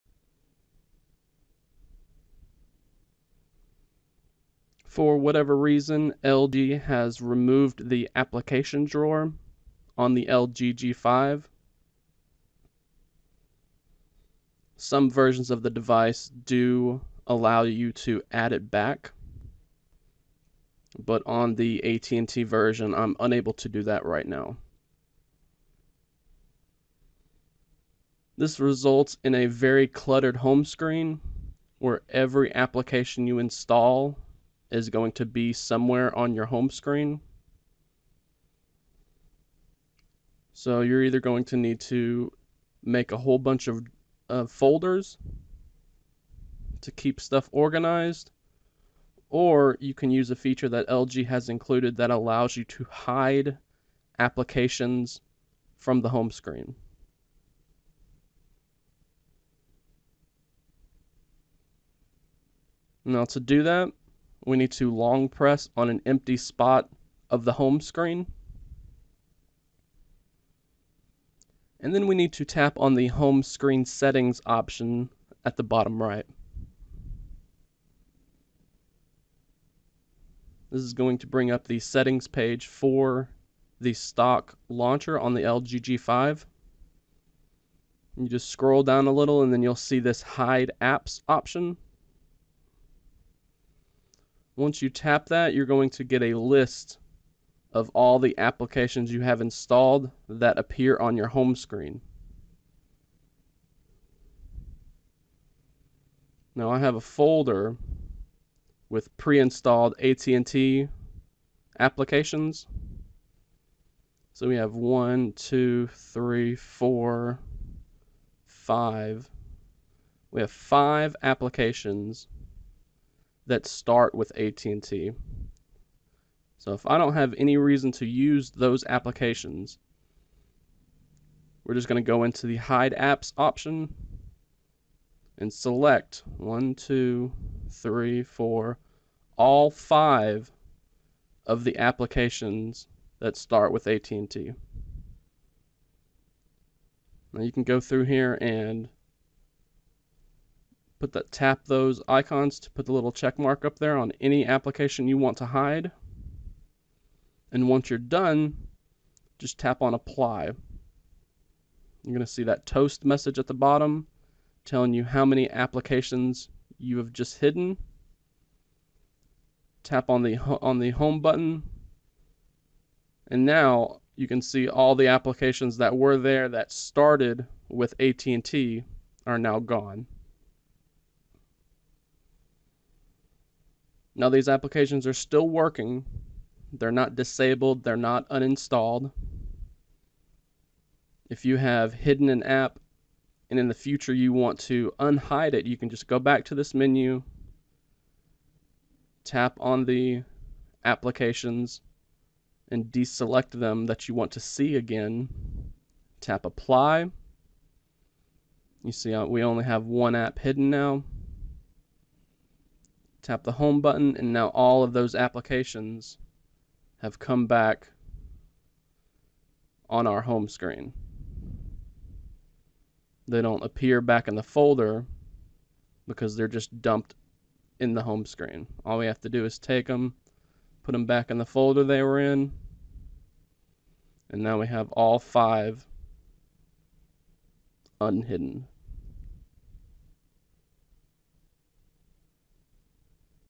LG G5 Hide Apps on the Home Screen
By default, LG removed the app drawer on the LG G5 so today I want to show you how to hide applications that appear on the home screen.